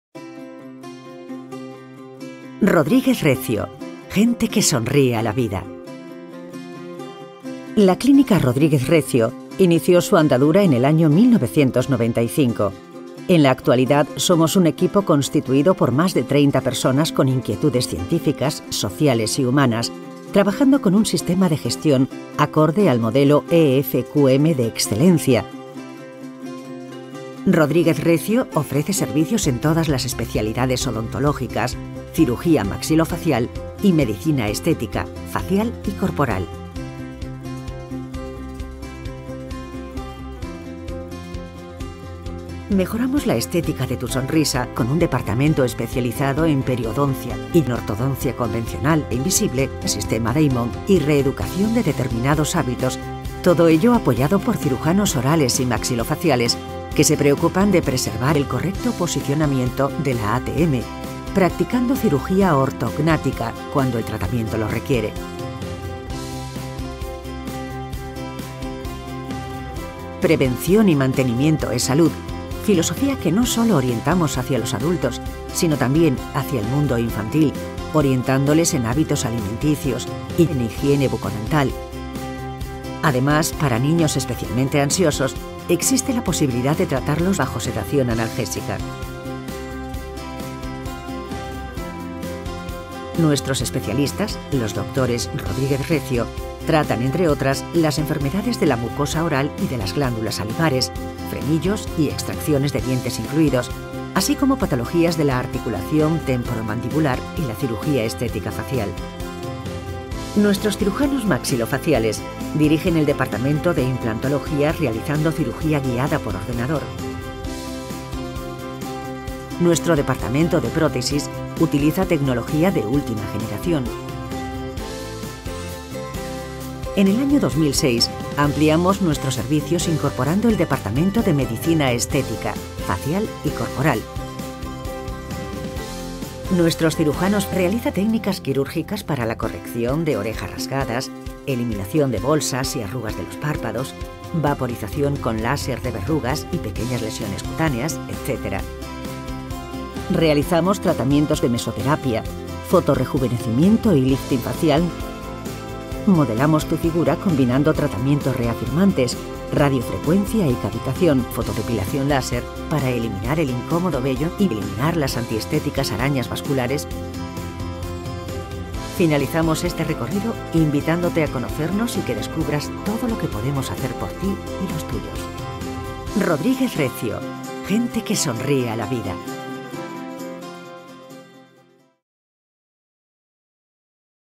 CLÍNICA RODRIGUEZ RECIO
Producción destinada a dar a conocer las distintas áreas de trabajo que se desarrollan en la Clínica Rodríguez Recio.